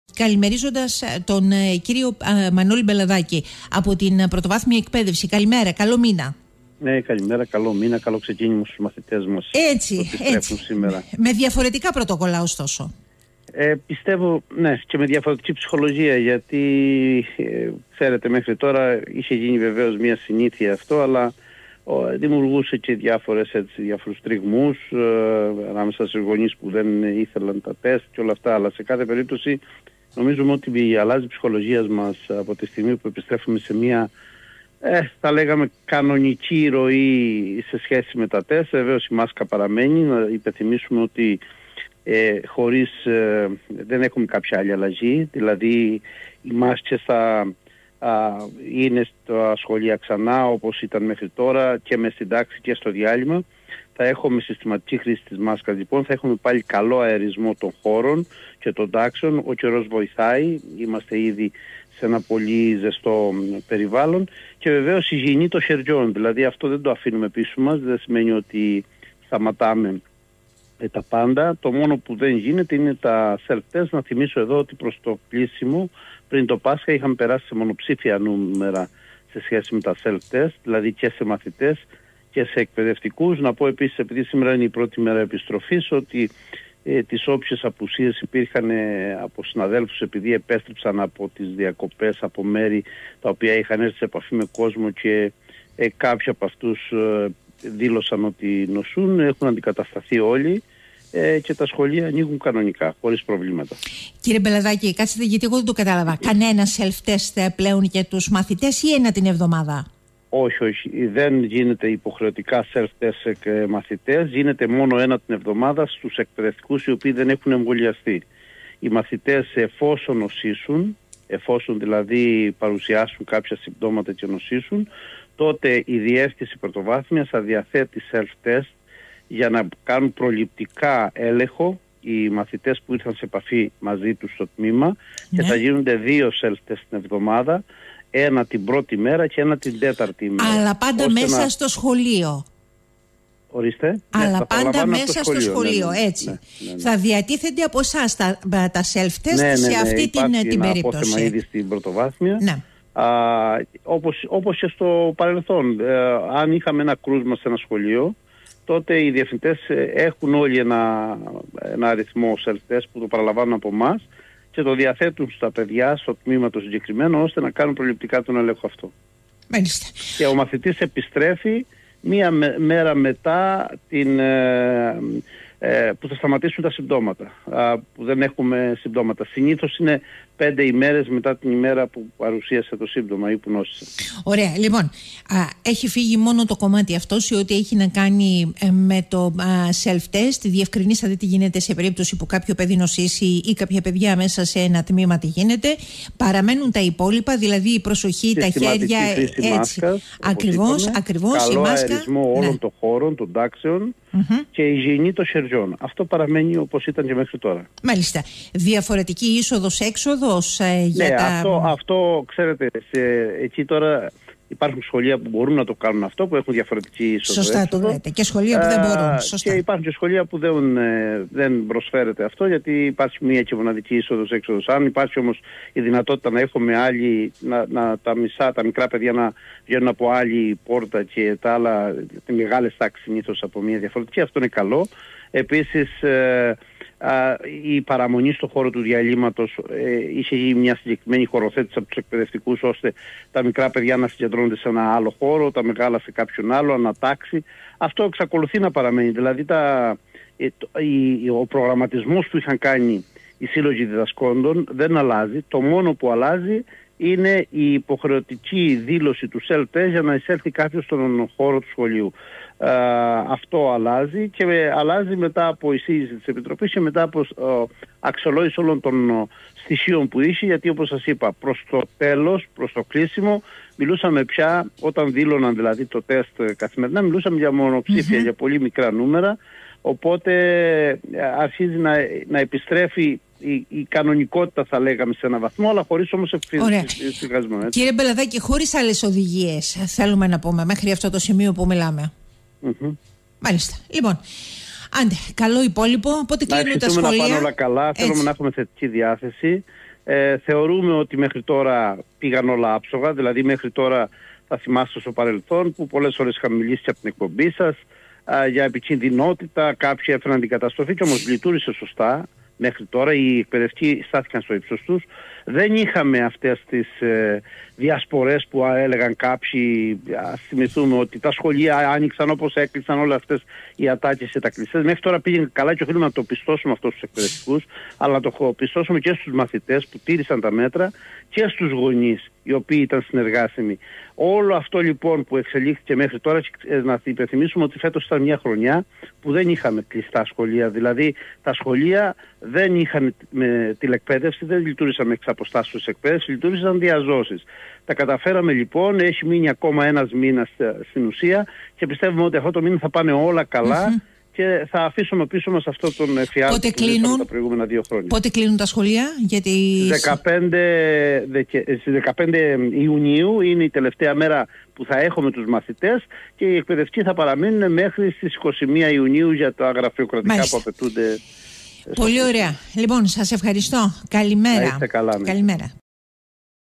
Μ. Μπελαδάκης: Με καλύτερη ψυχολογία σήμερα η επιστροφή στα θρανία
Παραμένουν τα υγειονομικά πρωτόκολλα περί ατομικής υγιεινής, μάσκας, χώρων

Με νέα υγειονομικά πρωτόκολλα ανοίγουν σήμερα Τρίτη (3/5) σχολεία, πανεπιστήμια, ΙΕΚ και κολέγια.
Μαθητές και εκπαιδευτικοί επιστρέφουν στα θρανία χωρίς self test.
Ωστόσο, διατηρείται η υποχρεωτική χρήση προστατευτικής μάσκας σε εσωτερικούς και εξωτερικούς χώρους, καθώς και η χρήση αντισηπτικών, ο τακτικός αερισμός χώρων, ο σχολαστικός καθαρισμός επιφανειών και εξοπλισμού και τα ξεχωριστά διαλείμματα.
Σε περίπτωση επιβεβαιωμένου κρούσματος κορονοϊού στην τάξη, ισχύει ό,τι και στο γενικό πληθυσμό, δηλαδή το κρούσμα θα παραμένει σε απομόνωση τουλάχιστον για 5 ημέρες, με ημέρα ανίχνευσης του θετικού τεστ να θεωρείται ως ημέρα 0.
«Μάσκες, καθαριότητα κι ότι άλλο είχαμε παραμένει στα σχολεία για την ασφάλεια όλων μας. Αλλάζει μόνο προς το καλύτερο η ψυχολογία προς το καλύτερο της μαθητικής κι εκπαιδευτικής κοινότητας» σημειώνει μιλώντας στον 9.84 ο Δ/ντής Πρωτοβάθμιας Εκπαίδευσης Μανόλης Μπελαδάκης.